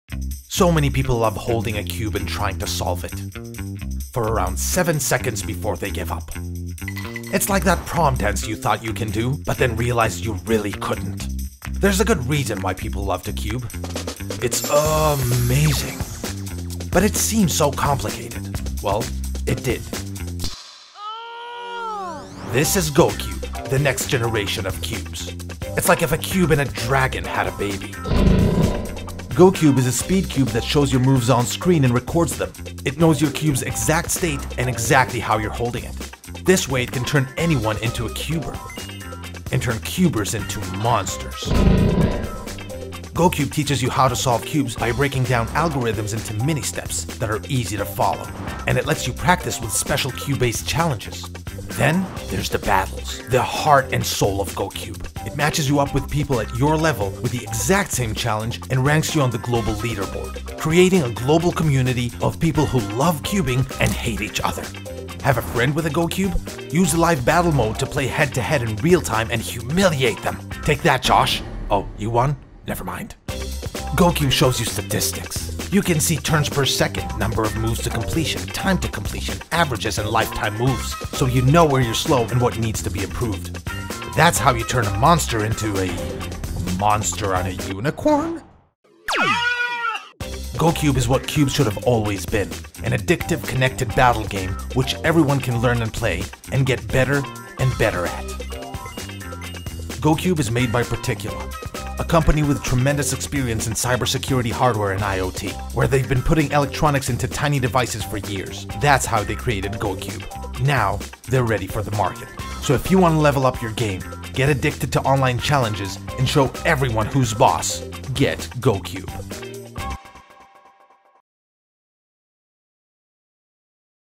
GoCube - Official Video
Particula has created GoCube, a 2018 smart app-connected spin on the classic game that reduces the frustration factor and helps turn anyone into a Cube Master from the comfort of home. The tech is equipped with sensors to track and measure activity, and an inertial measurement unit to track the cube’s position.

With tutorials for beginners and coaching for players looking to hone their skills, users can embark on missions to solve cubes, play the app’s prepackaged games, face off against friends, or compete fairly against players around the world. GoCube can even be used as a controller for other games.